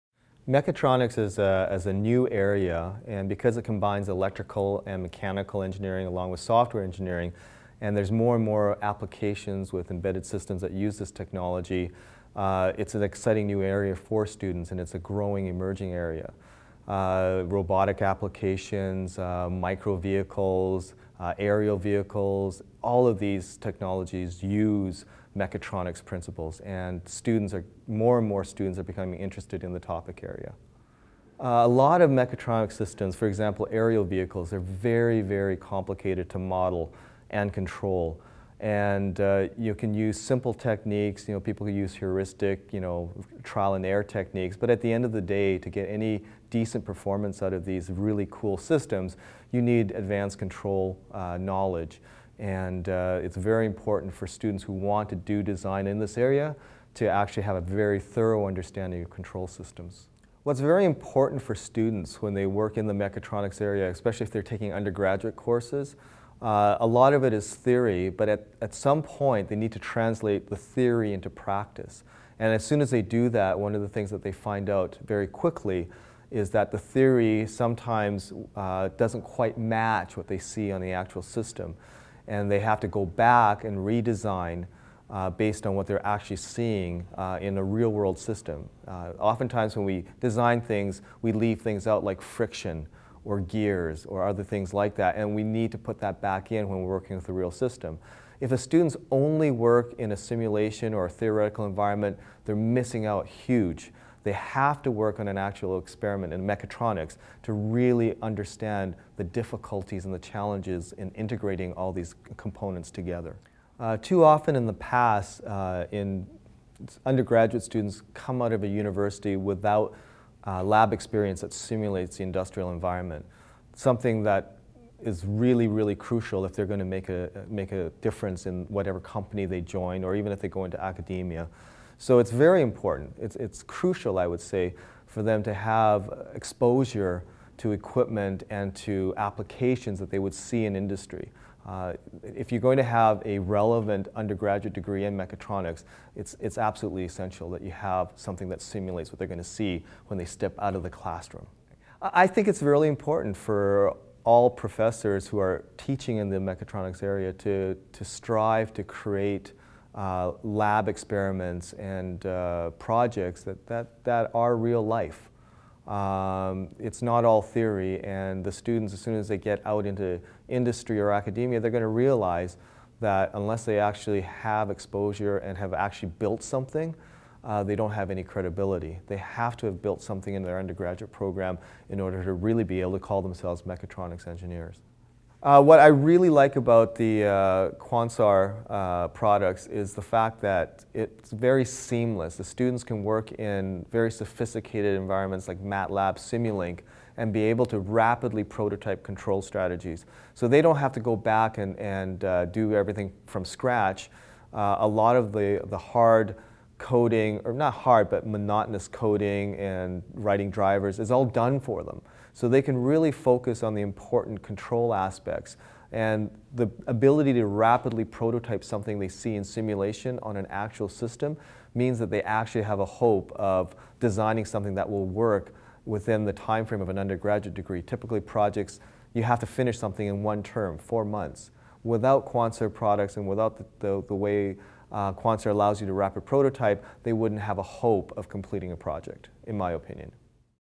Why Controls is Critical
Quanser talked to Dr. Wang on the role of controls in the undergraduate engineering education.

Dr. David Wang, Professor at the University of Waterloo's Department of Electrical and Computer Engineering, talked to Quanser about the role of control in mechatronics and robotics, and how Quanser systems and QUARC rapid control prototyping software help his undergraduate students to better understand and apply the concepts.